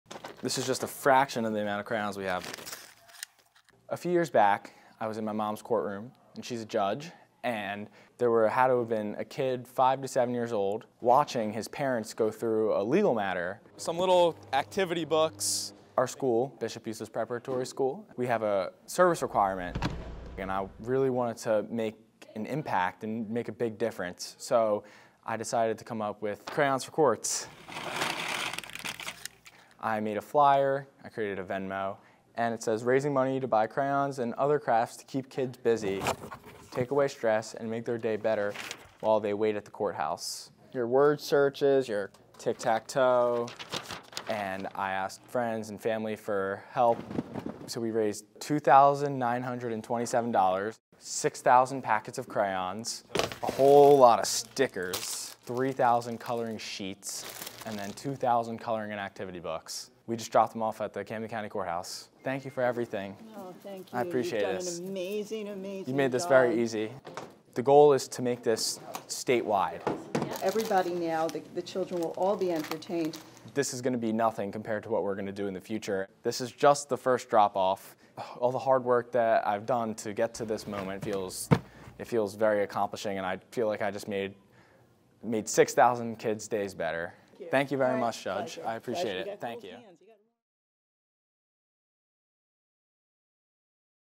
‘Crayons for Courts’: South Jersey teen makes life more colorful for kids in the courtroom
CRAYONS FOR COURTS: 17-year-old Andrew Passalacqua has a mission to make life more colorful for kids in the courtroom. 🖍️🏛️